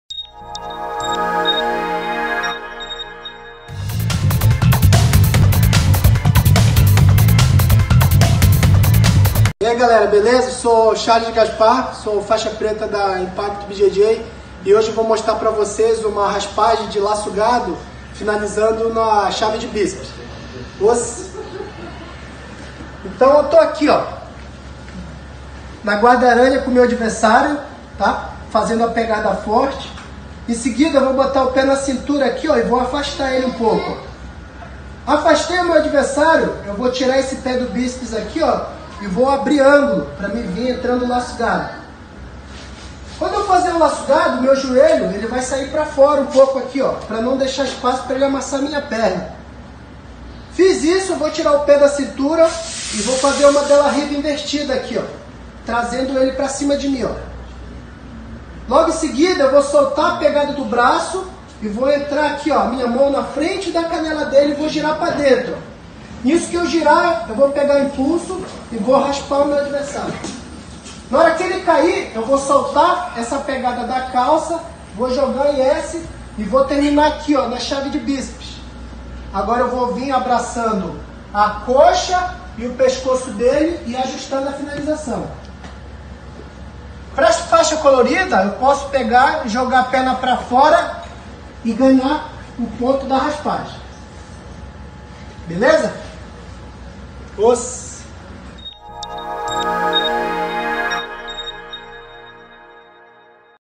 ブラジリアン柔術テクニック ラッソガードスイープ チャールズ・ガスパー Raspagem de Guarda Laço Chales Gaspar
Impacto Japan BJJの黒帯チャールズ・ガスパーによるブラジリアン柔術テクニック。
今回はラッソ―ガードからスイープして極めるまでのテクニックを紹介しています。

O instrutor Charles Gaspar Faixa Preta da Impacto Japan BJJ ensina como fazer uma raspagem da guarda laço.